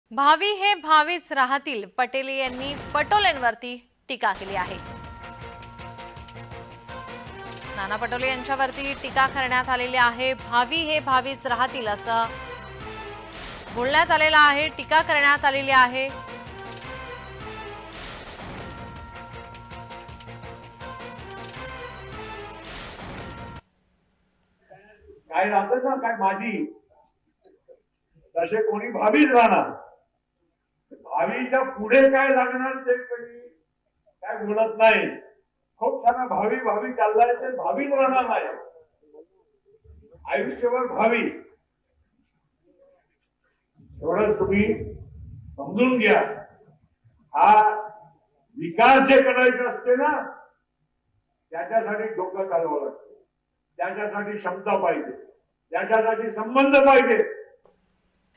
Praful Patel VS Nana Patole | 'भावी हे भावीच राहतील' | Marathi News
'भावी हे भावीच राहतील'
पटेलांची पटोलेंवर टीका